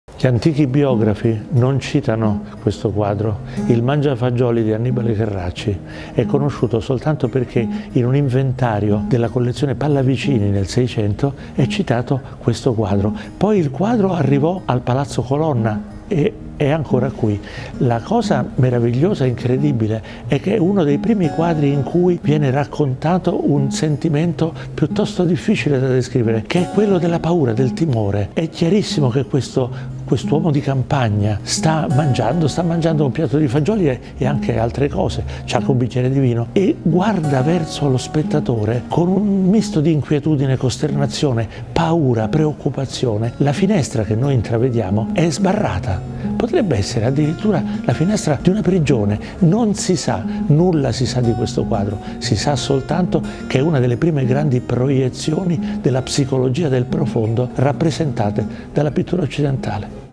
Annibale Carracci in 60 secondi
"I Maestri in 60 secondi" vi parla del Mangiafagioli di Annibale Carracci. Un capolavoro celeberrimo, a torto considerato un dipinto di genere. In realtà, un esempio di introspezione psicologica, anticipatore dell'arte contemporanea. Per voi, in 60 secondi.